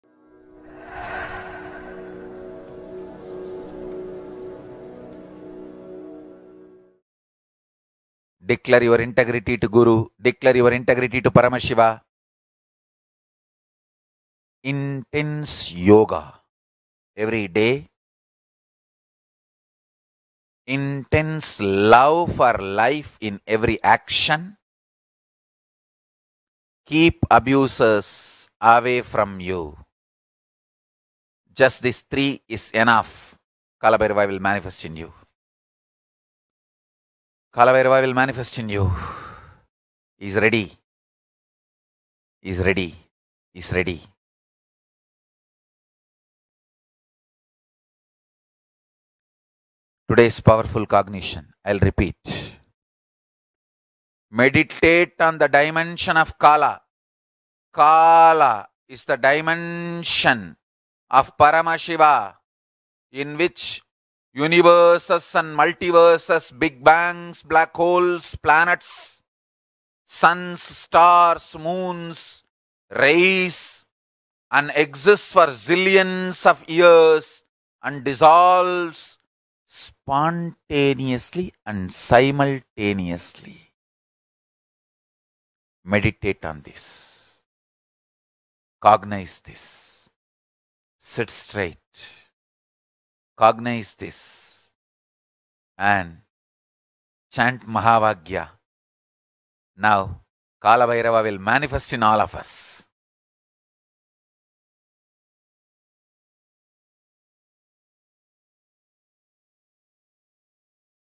Meditate on The Dimenson of Kaala
Declare your integrity to Guru, declare your integrity to Paramashiva, intense yoga every day, intense love for life in every action, keep abusers away from you, just these three is enough Kalabhairava will manifest in you. Kalabhairava will manifest in you, He’s ready, He’s ready, He’s ready. Today’s powerful cognition I’ll repeat: Meditate on the dimension of kaala, kaala is the dimension of Paramashiva in which universes and multiverses, big bangs, black holes, planets, suns, stars, moons raise and exist for zillion of years and dissolves spontaneously and simultaneously, meditate on this, cognize this. Sit straight, cognize this and chant Mahavakya now Kalabhairava will manifest in all of us.